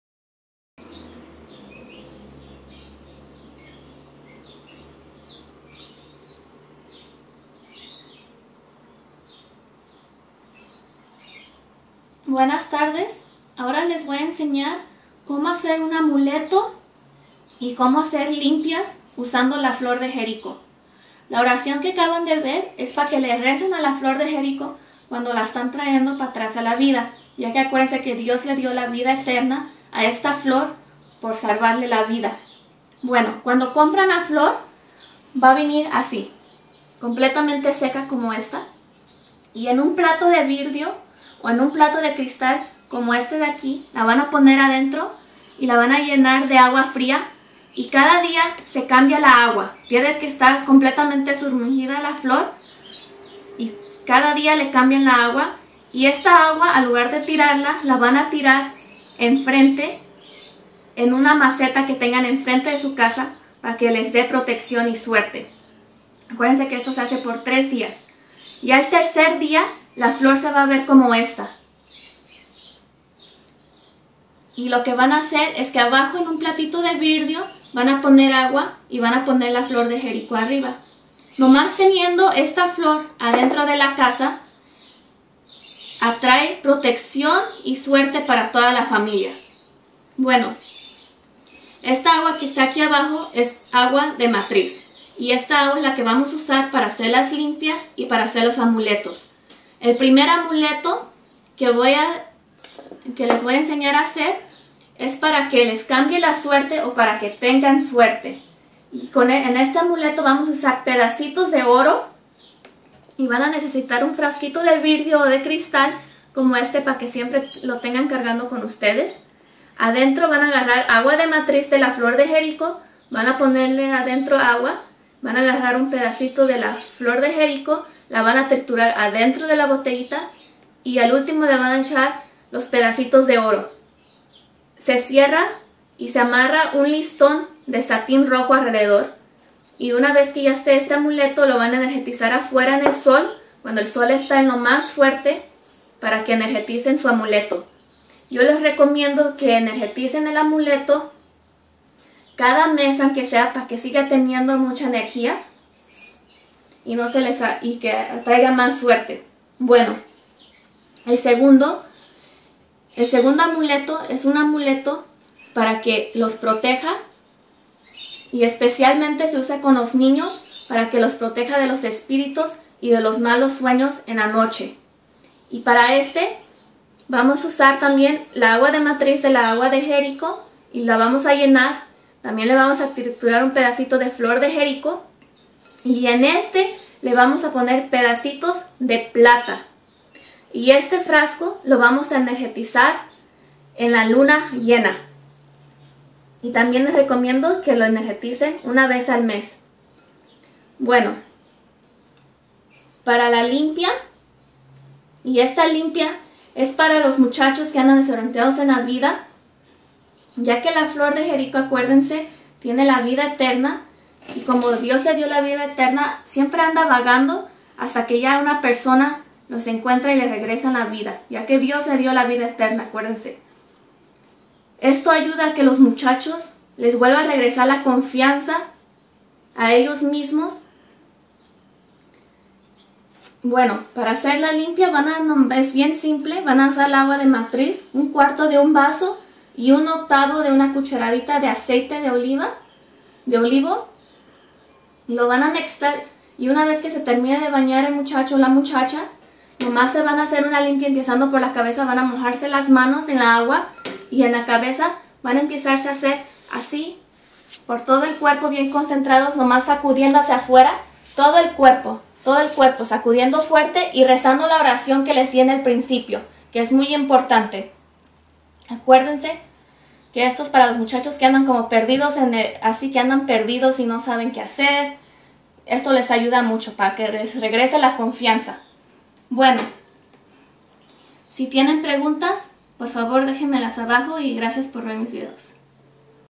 Flor de Jerico: Amuleto, Oracion, proteccion para la casa, y limpia.
"En el nombre del padre del hijo y espiritu santo seas bendita. Ya que tu socorristes a Dios dandole agua cuando estuvo en el desierto. En agradecimiento por salvarle la vida Dios te dio la vida eterna en una forma durmiente, y la virtud de protejer y atraer buena suerte a quien te encuentre en el camino y te ofresca agua para regresar a tu forma original. Amen~"

Reza esta oracion cada vez que le des agua a la flor.